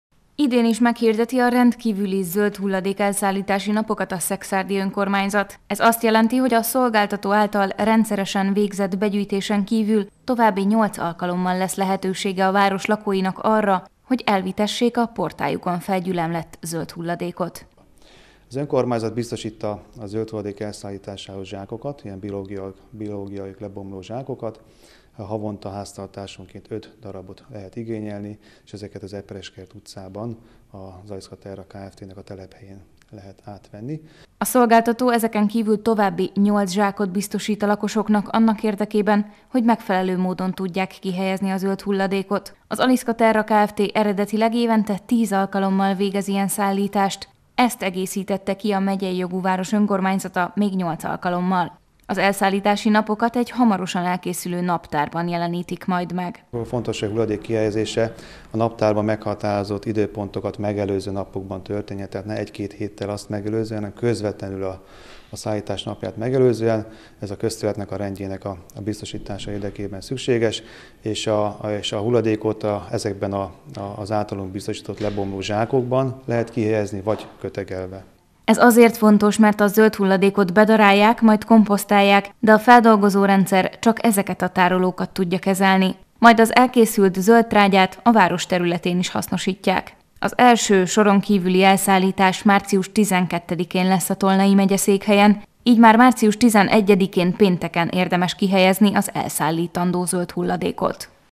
Rendkívüli zöldhulladék elszállítás Szekszárdon
Rendkívüli zöldhulladék elszállítás Szekszárdon - zsákban vagy kötegelve kell kihelyezni a leveleket, faágakat.

Március 12-én lesz az első rendkívüli zöldhulladék elszállítás Szekszárdon. A szolgáltató cég által biztosított évi 10 nap mellett a tolnai megyeszékhely önkormányzata újabb 8 napra szerződött a céggel. Varga András, a Városigazgatási és Rendészeti osztály vezetője, a legfontosabb tudnivalókra hívta fel a figyelmet.